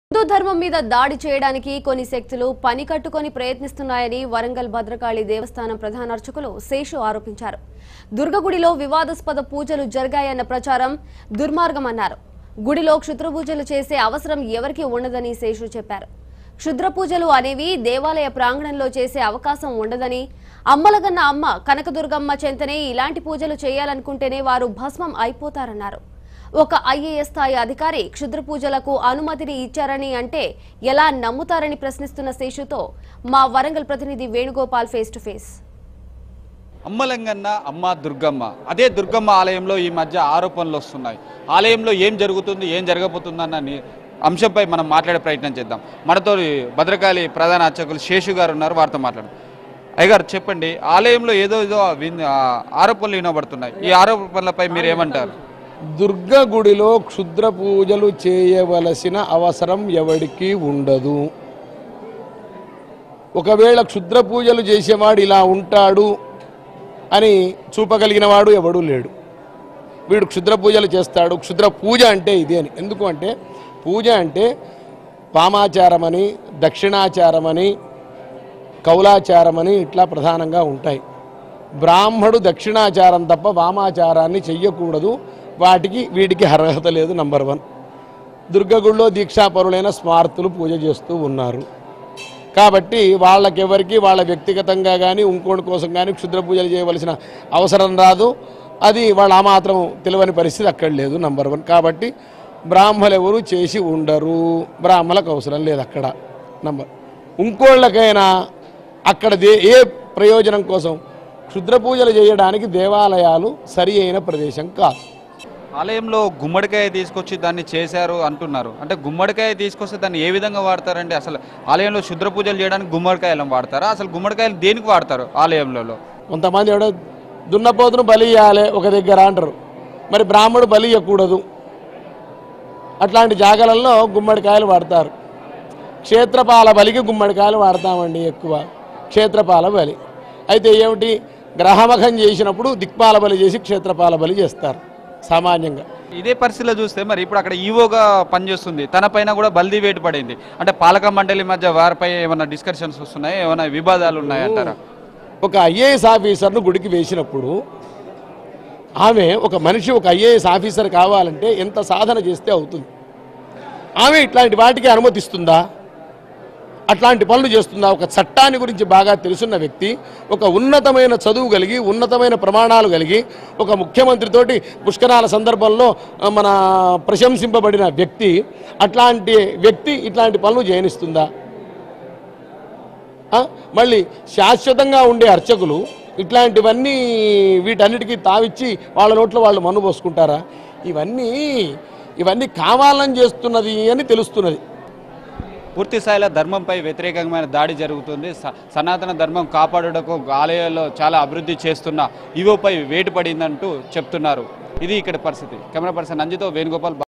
Bhadrakali Temple Priest Seshu Face To Face | Durga Temple Kartik pooja Conspiracy
Bhadrakali Temple Priest Seshu Face To Face | Durga Temple Kartik pooja Conspiracy | Bharat Today